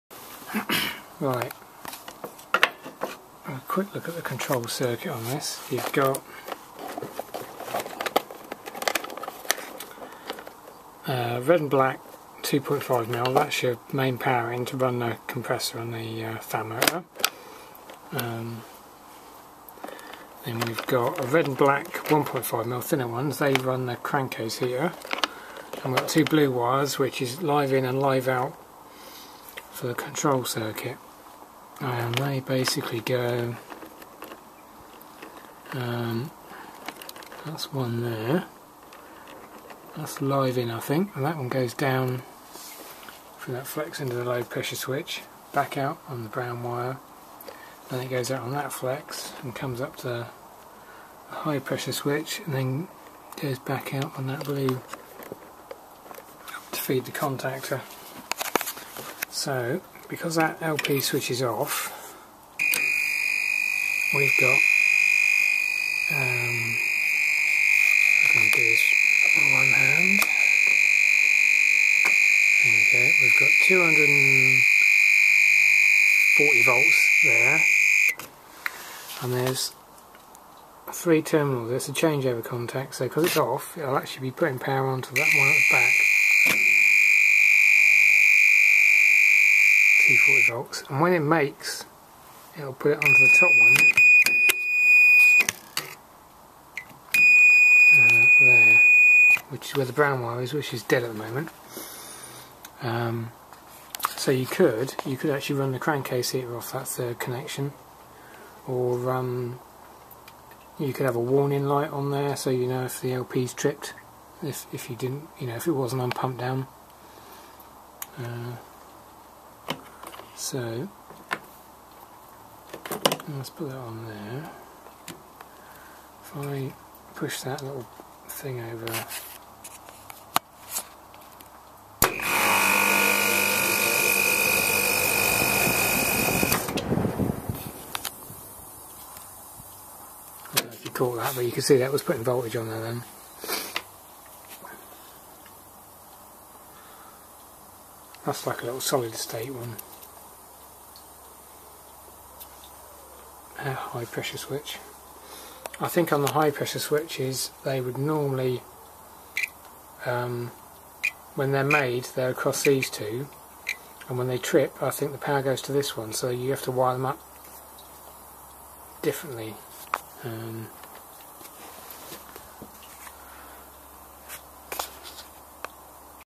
HVAC Low Pressure Switch
A quick look at an adjustable low pressure switches wiring.

This switch is fitted on a Copeland scroll unit cooling a milk tank on a farm. The system runs on pumpdown cut out so its this switch thats responsible for deciding when the compressor should start and stop once the solenoid valve has operated.

This switch has probably operated 20,000 times in its 15 years.